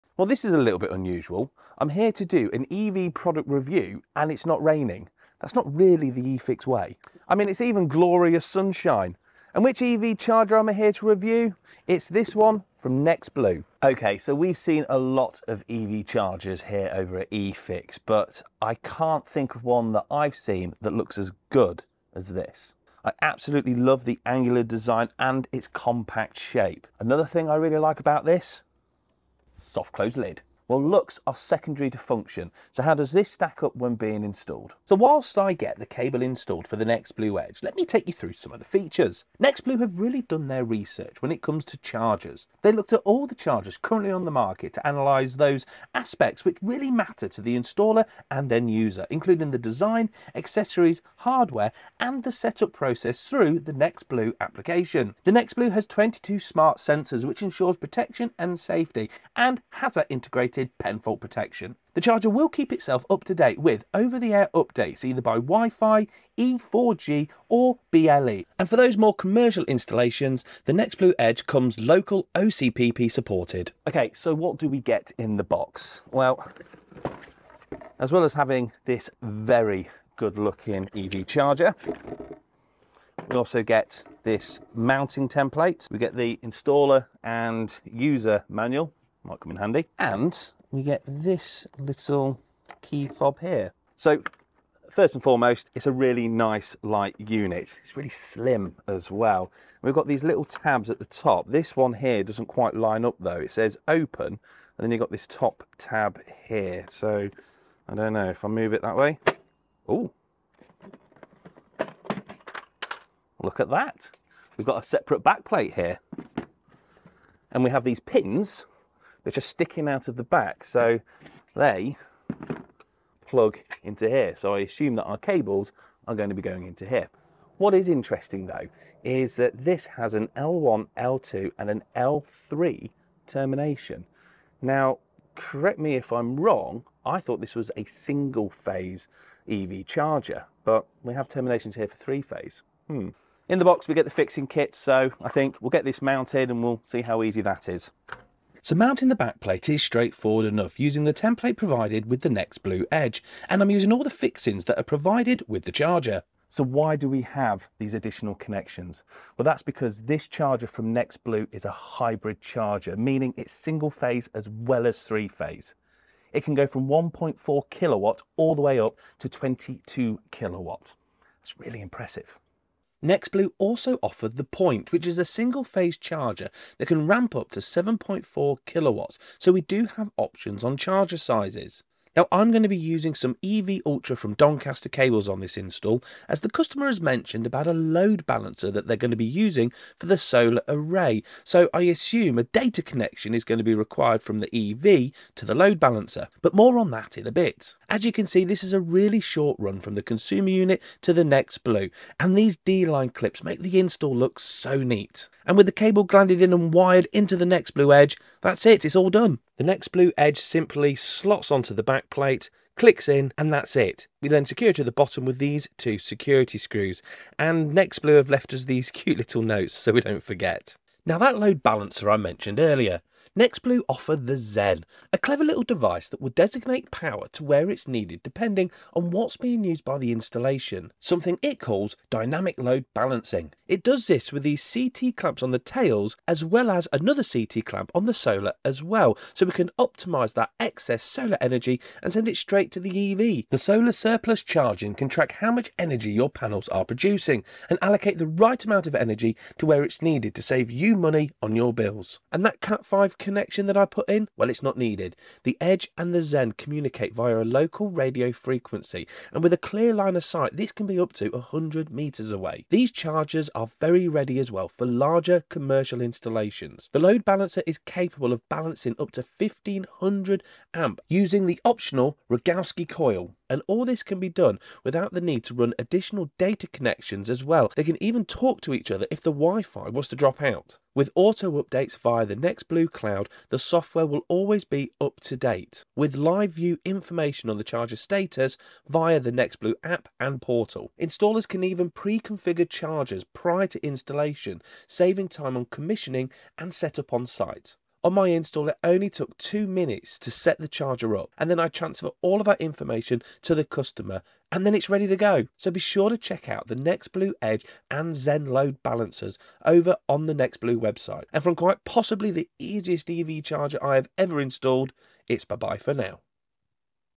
NexBlue Edge EV Charger: Is It Too Easy To Install?
Discover the NexBlue Edge EV Charger—a perfect blend of style and functionality! This compact, angular charger is not only stunning but also easy to install.

Packed with 22 smart sensors, advanced pen fault protection, and seamless updates, it’s built for safety and efficiency. With hybrid single and three-phase capability, dynamic load balancing, and solar integration via the Zen load balancer, NexBlue offers powerful features for home and commercial setups alike.

Time Stamps ⏱
00:00 An EV charger review without it raining?!?!
00:34 Is the NexBlue top easy to install?
02:07 The charger is single-phase & three-phase
02:29 Mounting is a doddle
03:40 The Zen allows dynamic load balancing
04:42 The easiest charger Joe has ever installed